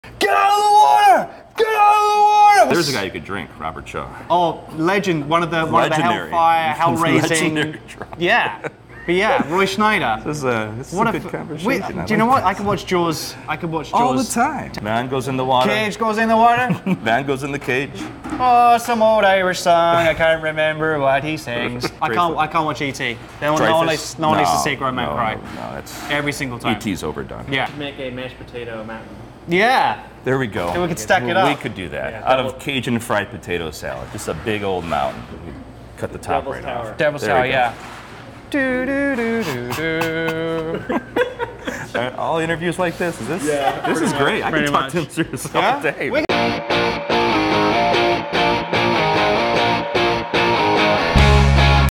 Chicago's Best Leftovers: Spielberg Love
An interview at Fatty's in Dekalb gets sidetracked.